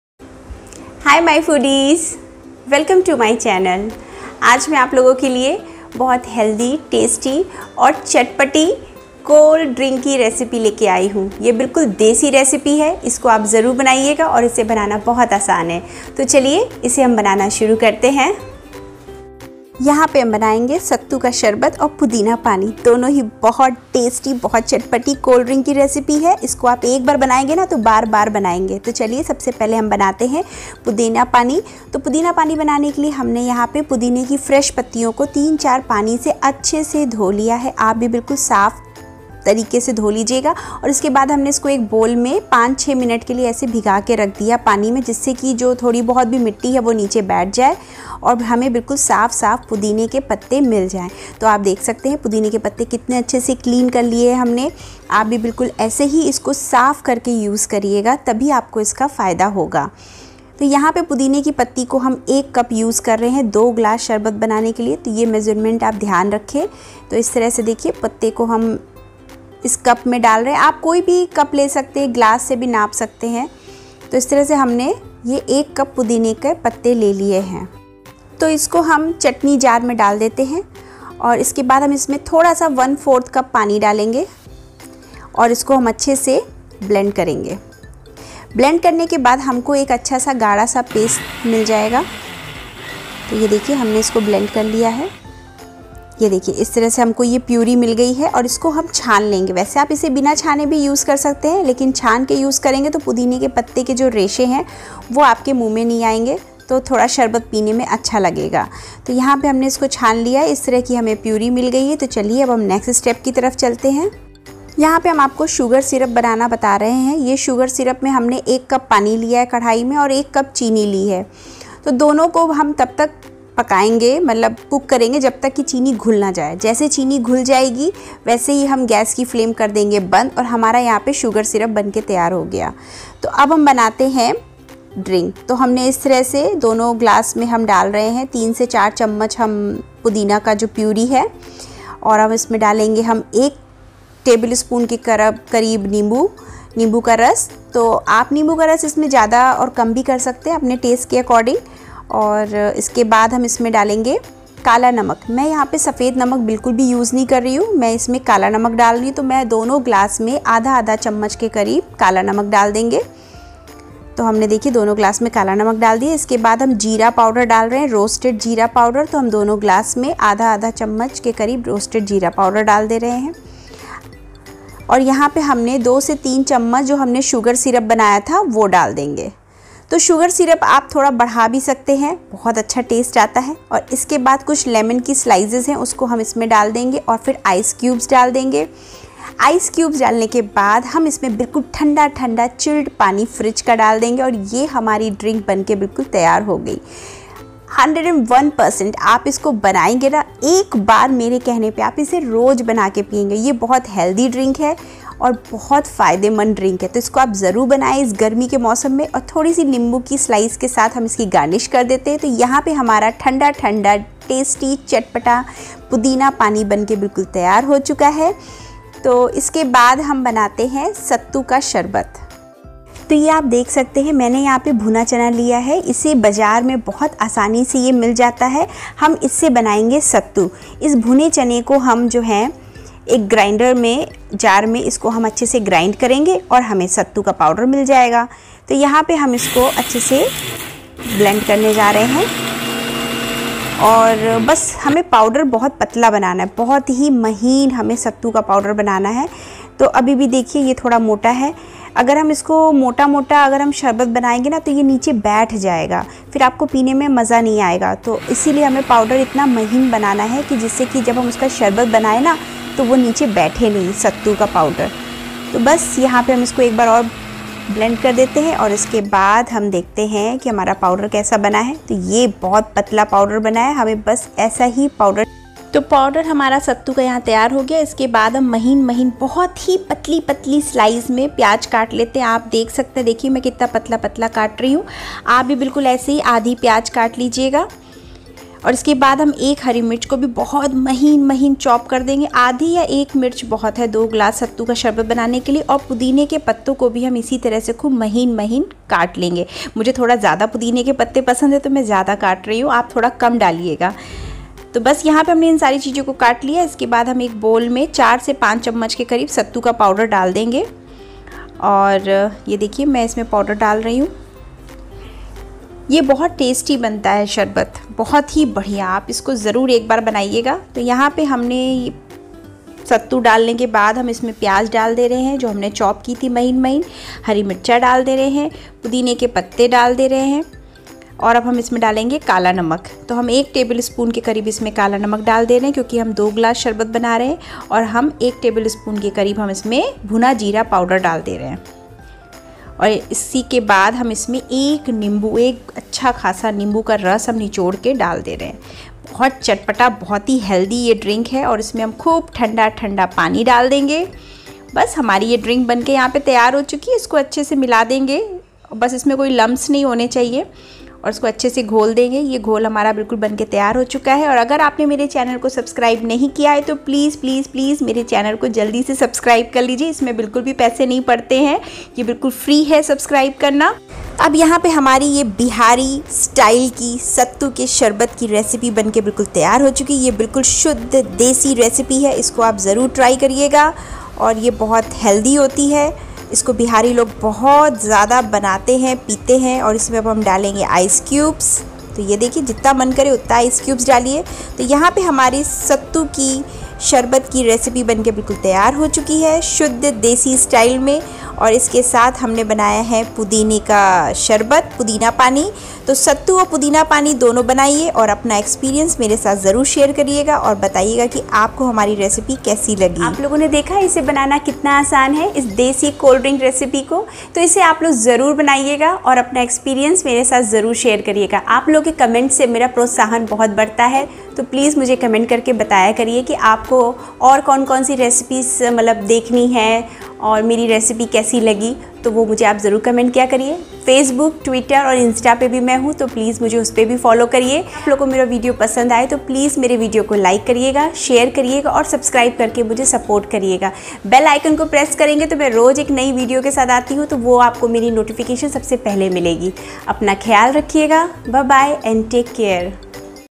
Sattu Sharbat , Pudina Pani - SummerDrinkSeriesByAshu - Part-3 | mint lemonade recipe
Hello Dosto, aaj hum log banaege Bihar ka famous Sattu Sharbat and Pudina Pani.
Dono hi recipe dil aur dimag dono ko refresh kar degi aur health beneficial bhi hai .